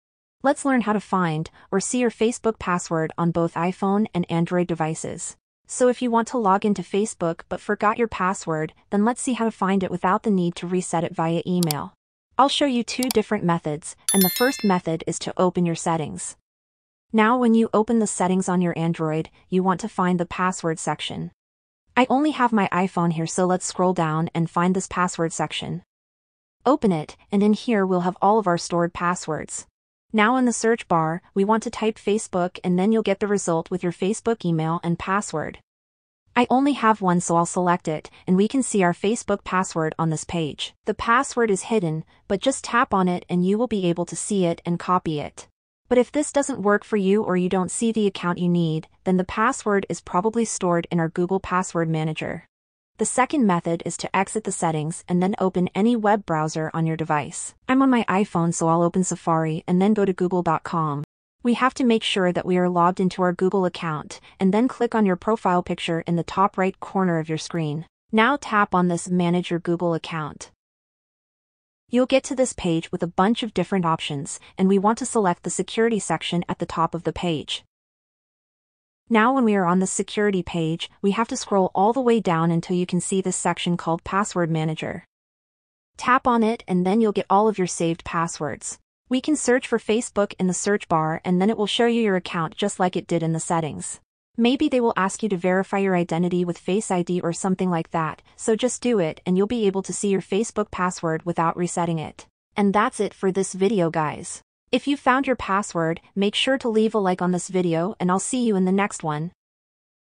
How to See Facebook Password - Full Guide
Let’s learn how you can find and see your Facebook password. If you forgot your Facebook password but want to login without resetting the password, this video is for you. This works on both iPhone and Android devices so just follow my steps.